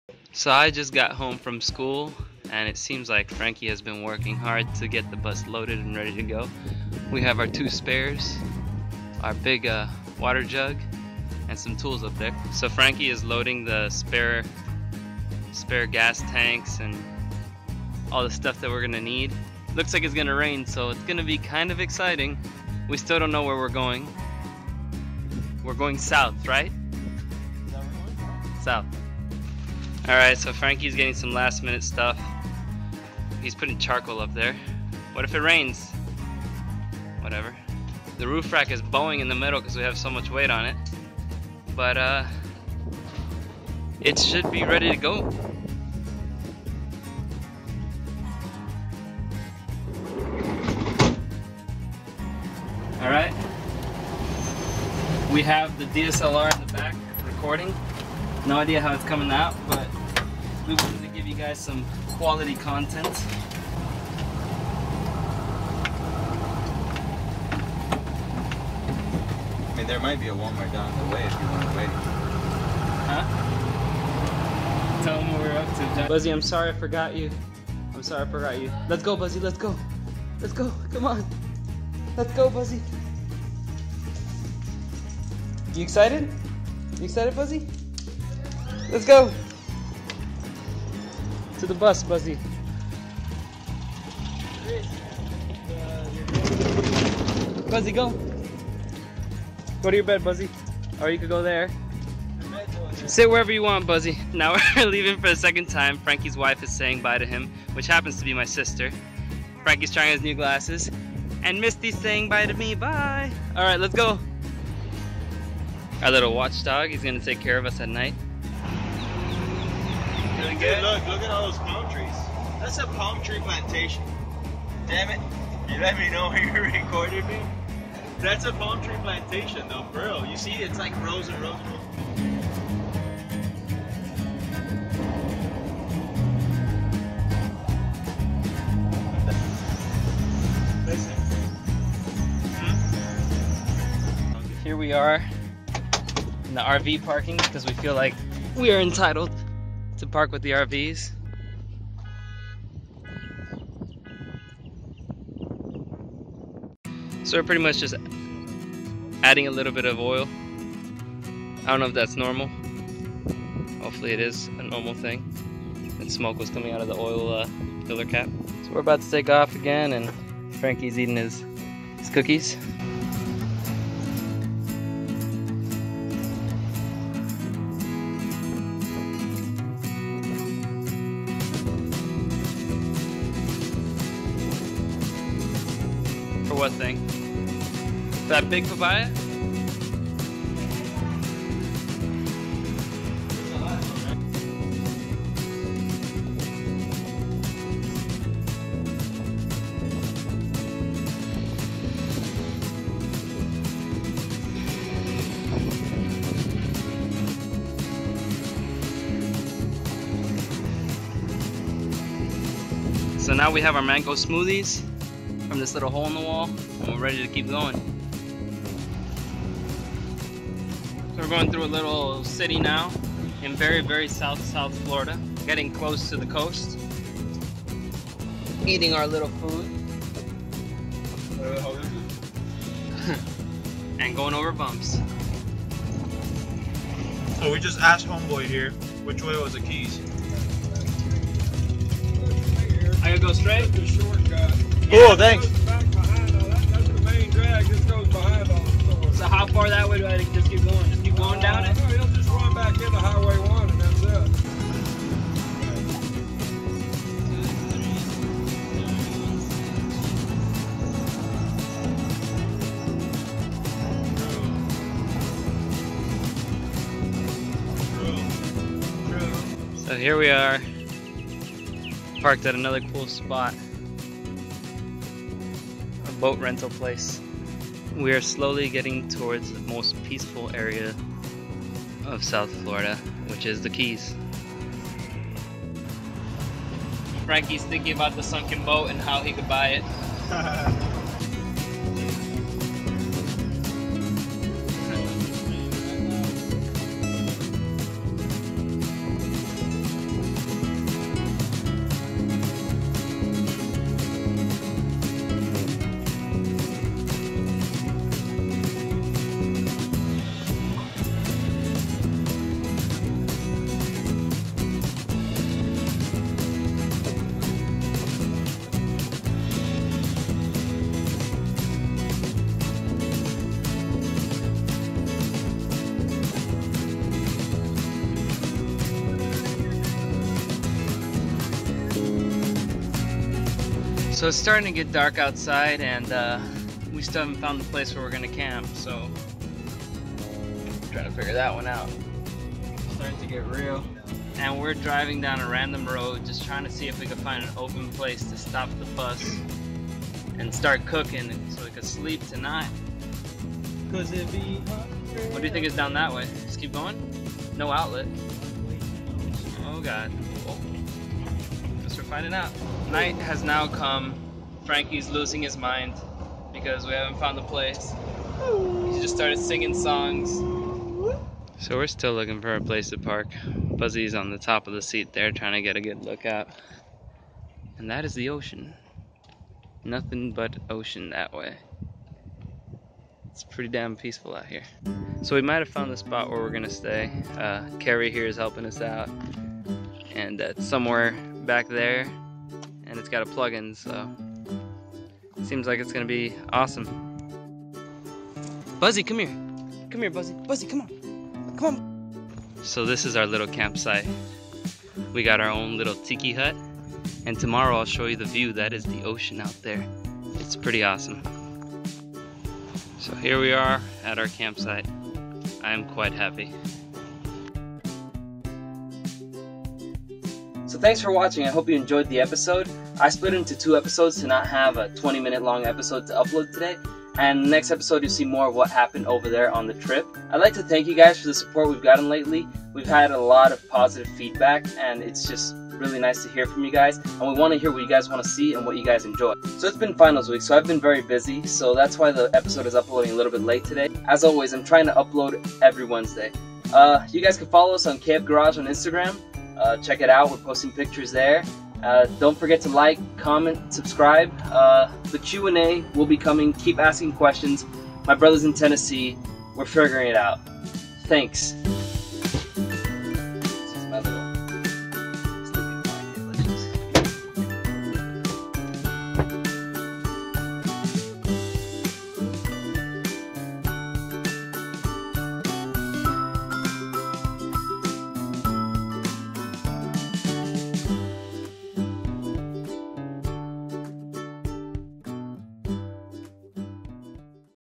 VW Bus (1973 TinTop) Camping Part: 1
In this episode we take the bus on its first road trip. We drive a few hours south on the Florida coast to go camping. Hope you guys enjoy :)